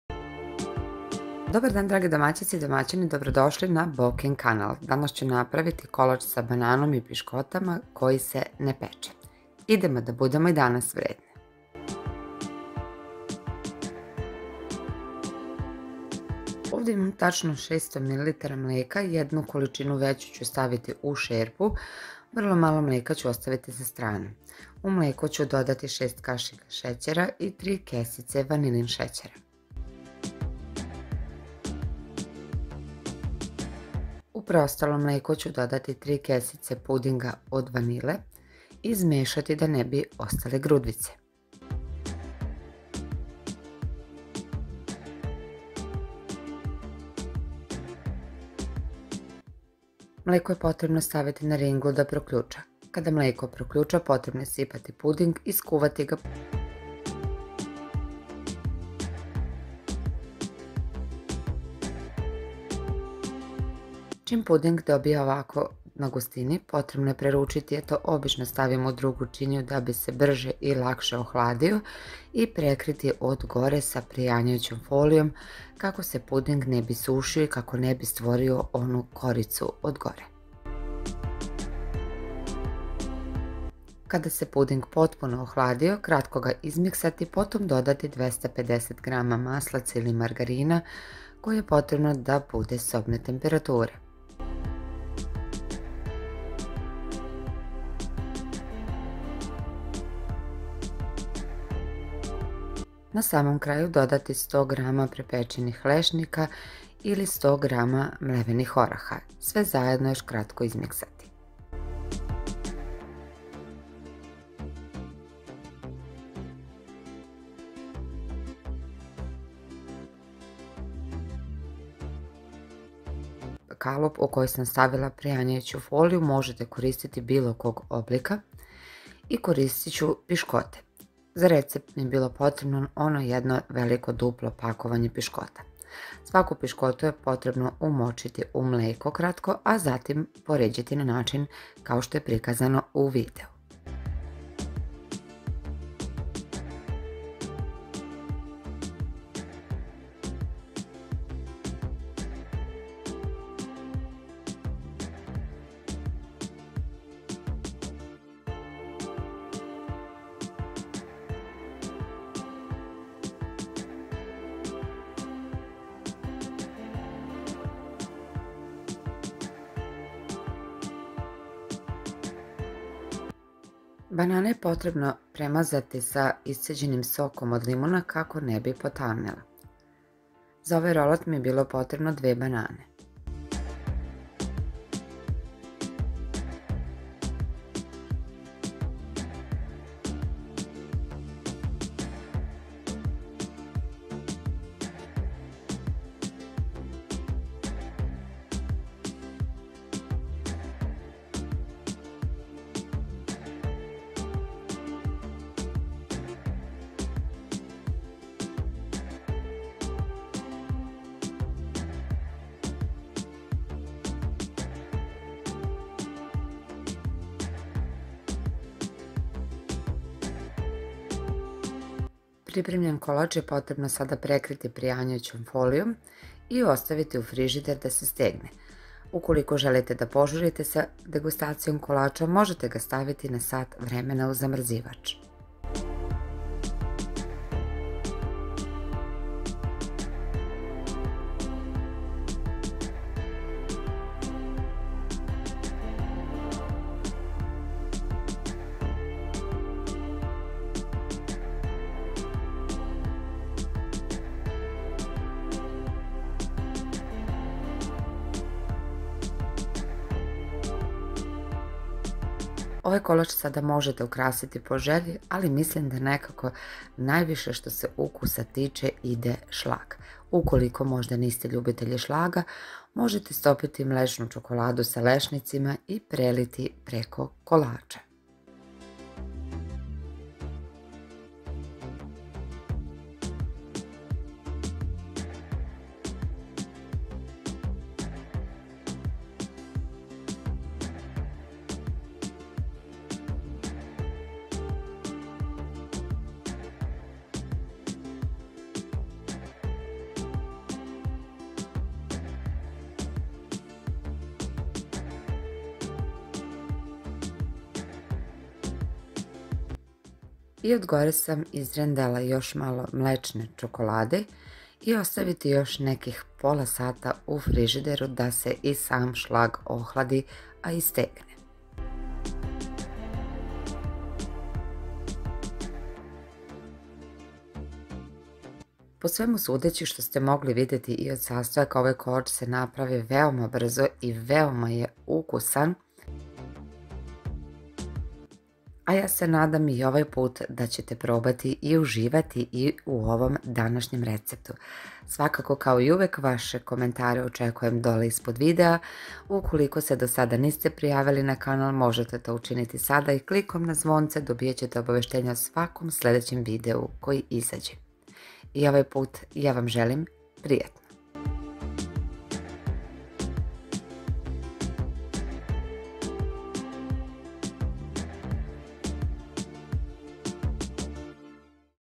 Kolac sa bananom,vanila filom i piskotama koji se ne peče
1 duplo pakovanje piškota
fil:
600 ml mleka
3 vanilin secera
3 pudinga od vanile -120 gr
6 kasika secera
250 gr margarina
100 gr oraha
2 banane
glazura
oko 100 gr slaga